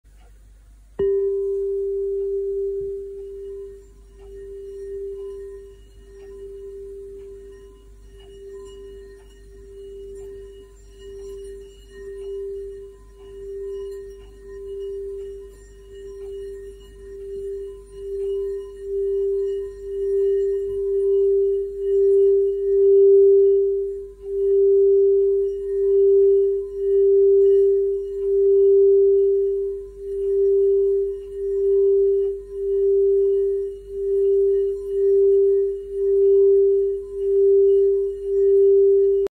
Throat Chakra
Crystal Bowl Throat Chakra
Short Crystal Bowl healing for the Throat Chakra - Speak your Truth